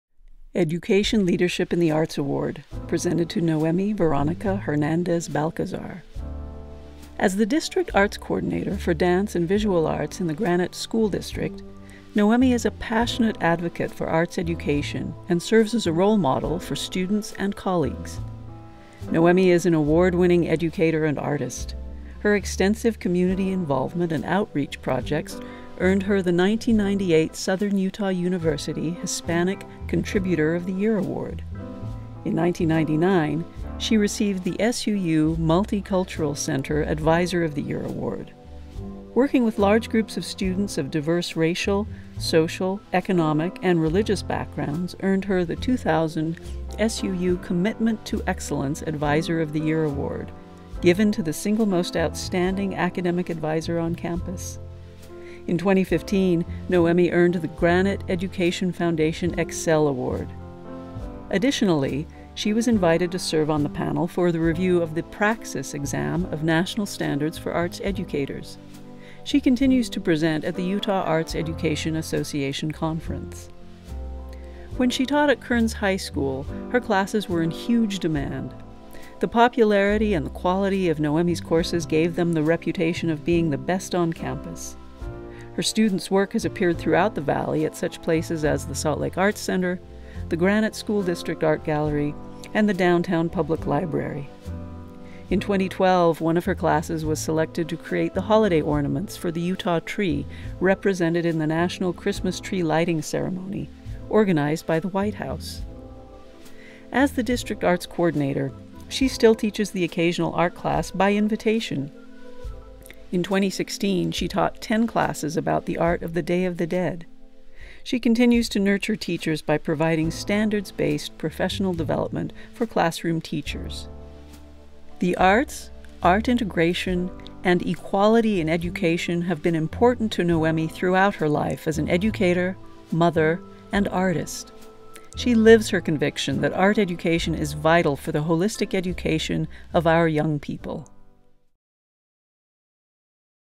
Noemi Veronica Hernandez-Balcazar
As the District Arts Coordinator for Dance and Visual Arts in the Granite School District, Noemi Veronica Hernandez-Balcazar is a passionate advocate for arts education and a role model for students and colleagues. An award-winning educator and artist, Noemi is committed to introducing her students to new ideas and providing standards-based professional development for teachers. The arts, art integration, and equality in education have been important to Noemi throughout her life as an educator, mother, and artist.